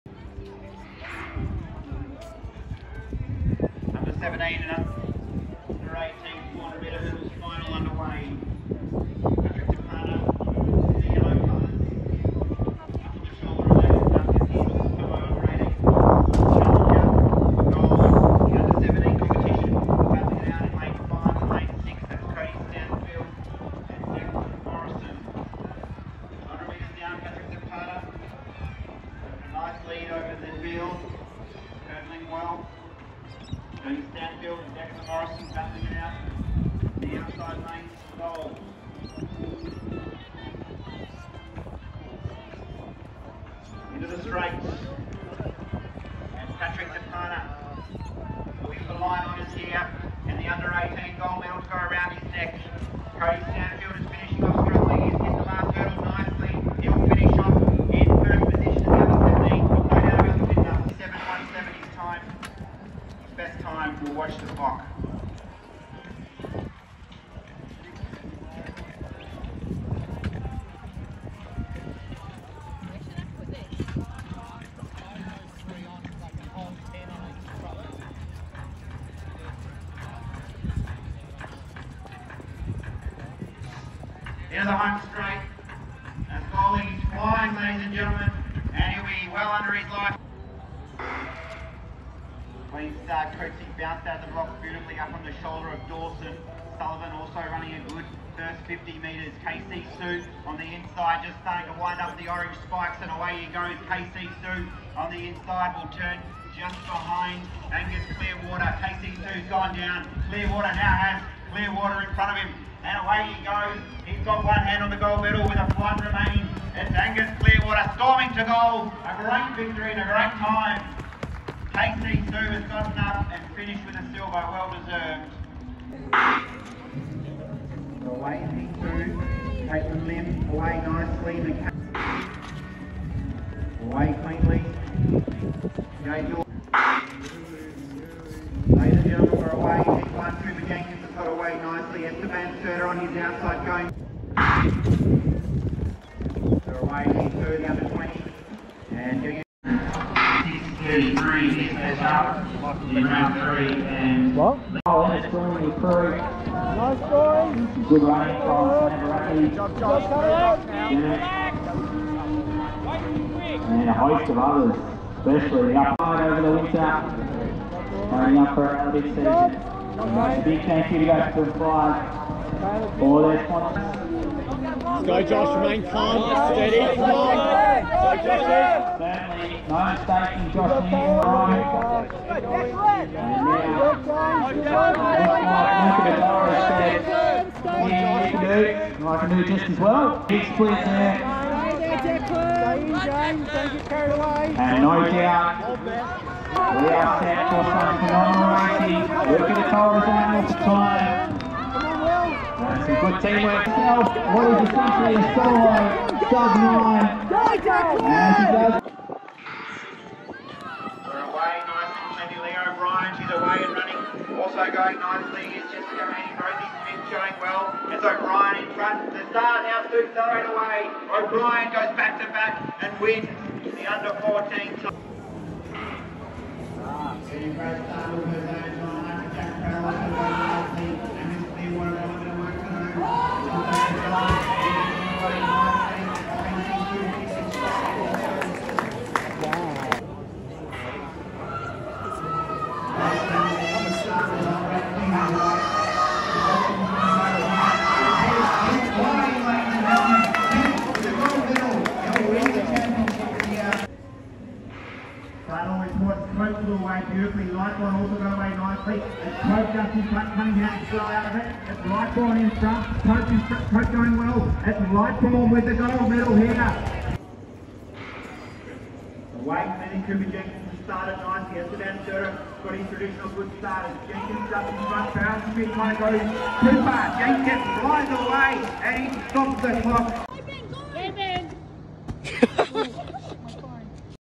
2021 WA State Championships: Day One
Day One Results:

Cooper Jenkins U20 100m🥇10.86
Cody Stanfield U17 400mH🥇58.87 (PB)
Kayel Ascoli U16 200mH🥇27.33
Angus Clearwater U15 200mH🥇27.85 (=PB)
Gabriel Doolan U18 100m🥈11.09
Declan Somers U16 3000m🥈9:00.69 (PB)
Jack Hodgen U16 100m🥈11.59 (PB)
Jordan Hamilton U15 100m🥈11.94
Daniel Dawson U14 100m🥉14.49 (PB)
Sharni Wimalasinghe U14 100m 4th 13.42 (PB)
Angus Clearwater U15 100m 4th 12.03 (PB)
Lubin Benedict U20 100m 4th 11.27 (SB)
Zac Wakefield U15 3000m 4th 11:06.60
Katie Mobilia U20 Javelin 4th 27.02m
Alexandra Tita U18 100m 4th 14.63
Thomas Dawson U15 200mH 5th 32.10 (SB)
Soha Abdelrahim U15 3000m 6th 11:23.74 (SB)
Archie Prall U15 200mH 7th 34.36
Tilly Duncan U14 100m 7th 15.34
Archie Prall U15 100m 7th 13.44
Thomas Dawson U15 100m 8th 13.56
Tyron Maloney U18 400m 9th 54.80 (PB)
Eva Philips U16 100m 10th 13.76 (SB)

Coach Catley:
@instagram.com/coach_catle...

West Track Club:
@instagram.com/westtrack_c...

Edited by Benjamin Catley